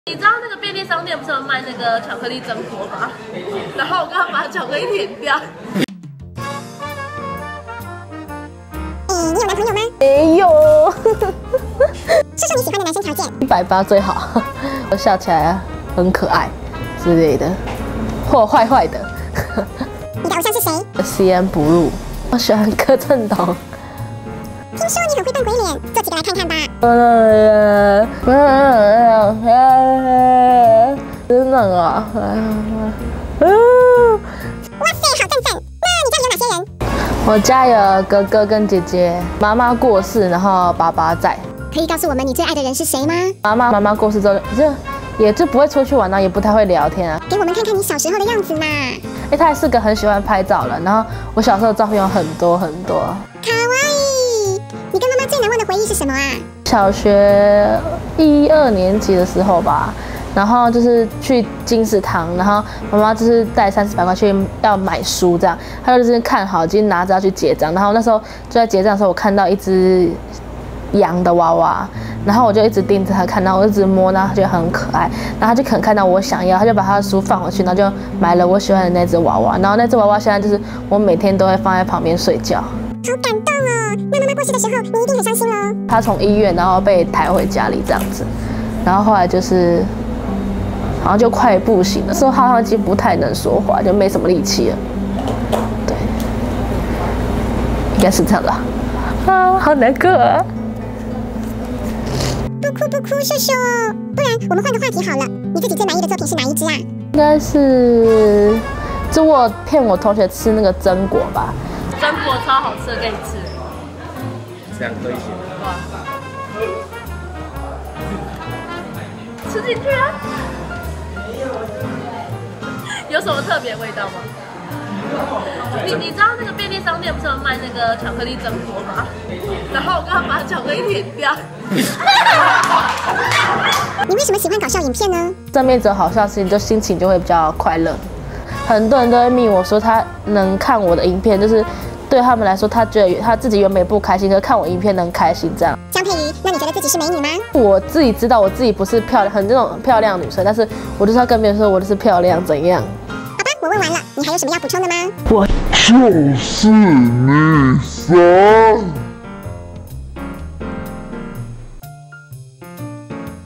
【台灣壹週刊】諧星背後的淚水…網路紅人江佩諭不為人知的身世
6個月前，我上傳一支搞笑影片〈同學請你吃榛果〉，沒想到蠻受歡迎。在那之後，我又陸續拍了不少影片，記錄生活中好笑的事，因此變成網路紅人。

很少人知道，我媽在我小三那年就癌症過世了。我是老么，跟兄姊差8歲，媽媽非常疼我，有次我跟她去書局，她帶了3、4百元要買書，正要結帳時，我看見一隻好可愛的羊布偶，結果媽媽把書放下，買了那隻羊布偶給我，它直到現在還每天陪我睡覺。以前我媽常帶全家人去釣魚、爬山，但她走後，家裡氣氛冷冷的，再也沒一起出遊。我回家總是直接進房，不太說話。

為何會變這樣，我不知道，但我想，在家裡不開心，至少在外面要開心，所以我扮醜、搞笑，努力成為同學的開心果。國中時，我暗戀一個男生，後來他跟我一個女生朋友在一起，我心裡很失落，卻表現得毫不在意，繼續搞笑。我最常開的玩笑就是：我最漂亮，怎樣！然後大家笑我、罵我，變成一個開心的互動。我覺得，身邊有好笑的事，就能比較快樂。

我知道我不漂亮，也想減肥，也想談戀愛啊，但就是懶得動。奇怪的是，帥哥很愛跟我做朋友，他們說相處起來好輕鬆，可以練肖話。我雖然沒男友，可是身邊很多帥哥，別的女生會羨慕，我也有小小的成就感。

我的臉書粉絲有30多萬，喜歡我的人會密我說，他原本不開心，可是看我的影片能開心。也有人嫌我又醜又胖，我一開始會介意，不過，我就是做自己才受到歡迎嘛。所以…我就是胖，怎樣！

※行動閱讀請下載《台灣壹週刊》APP
●iOS 免費下載

※更多社群媒體及時互動
●台灣壹週刊 | Facebook